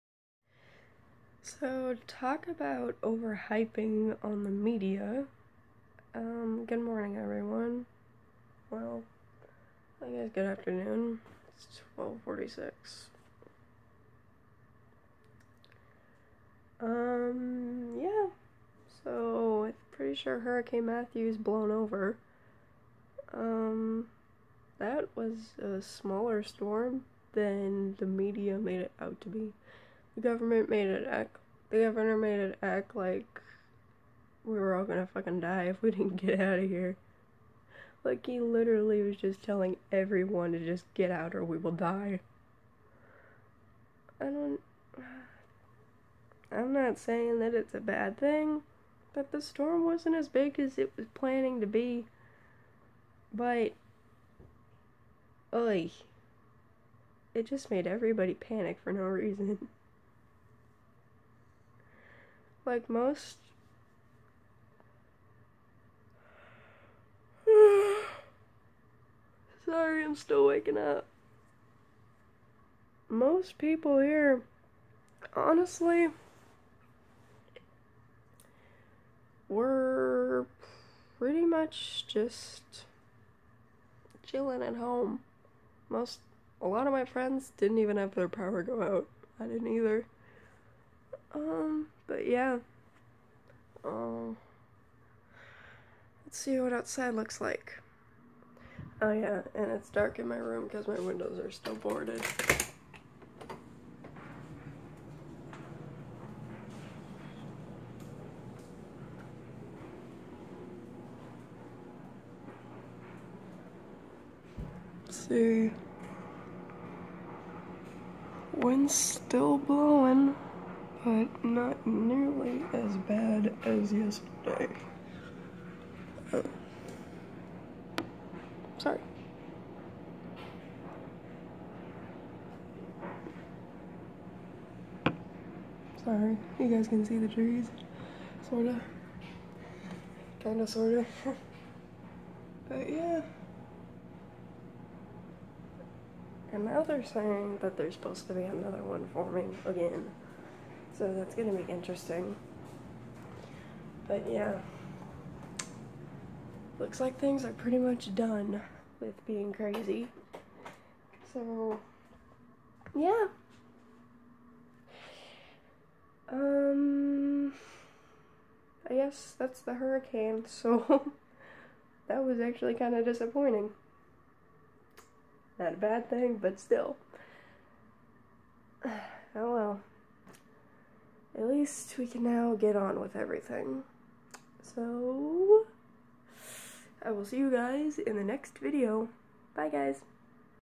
Hurricane Matthew 2016 | Day 2 (Was that all?)
This storm was so overly exaggerated on the news. But I am thankful for that honestly.